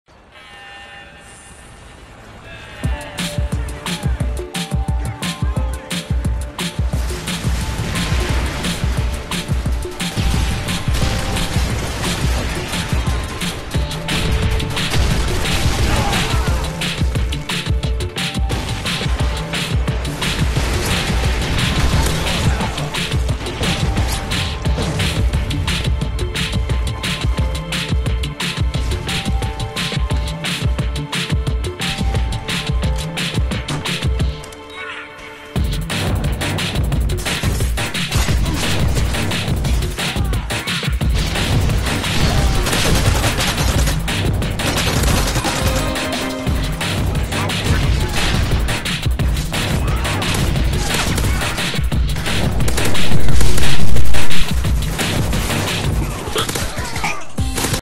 Inasuh - Epic C4 Multikill & Gunplay
Bustin' some butts on Dome. Starting with an epic quad C4, then I creep up on an enemy to exact revenge, followed by assorted gun on enemy action. I'm diggin' the camera work in this vid. Hopefully you do too.